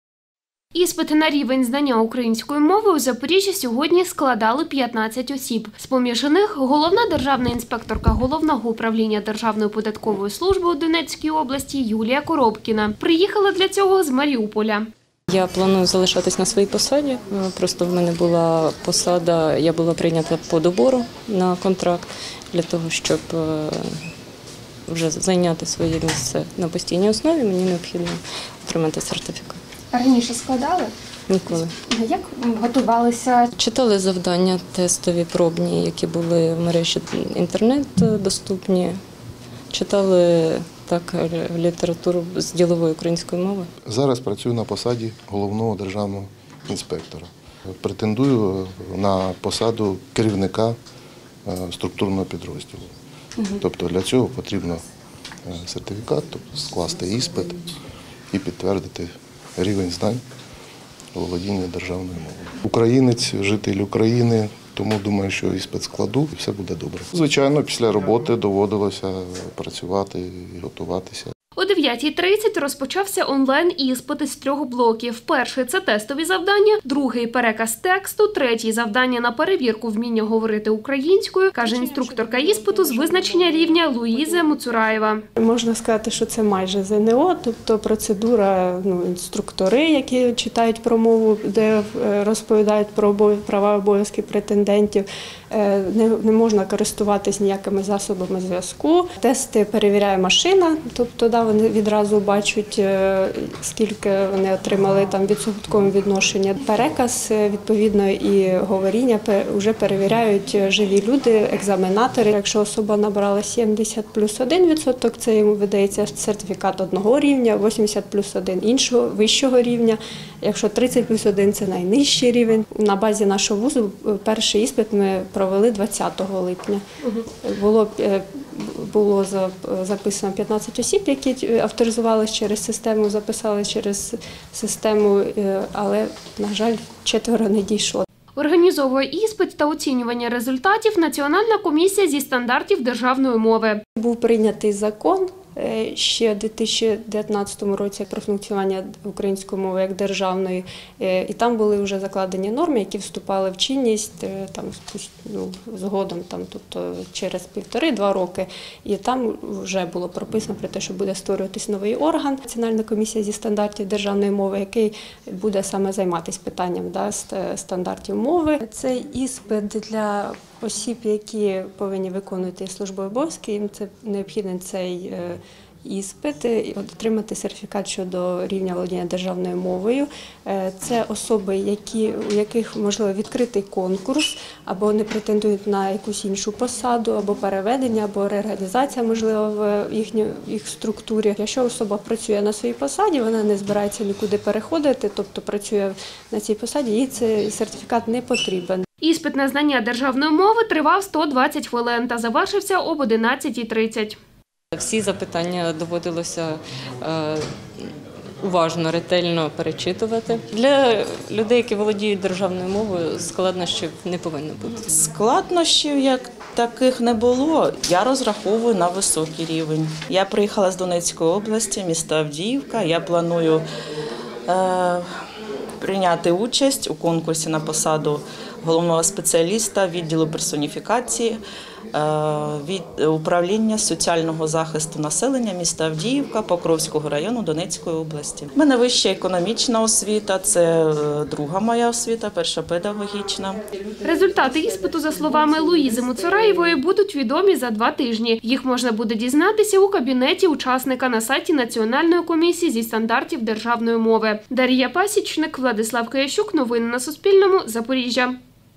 Держслужбовці в Запоріжжі складали іспит на рівень знання української мови | Новини
Державні службовці та ті, хто планує обійняти посади в органах місцевого самоврядування, сьогодні складали іспит з української мови за новим форматом. Відбувся він у 6 корпусі Запорізького Національного університету.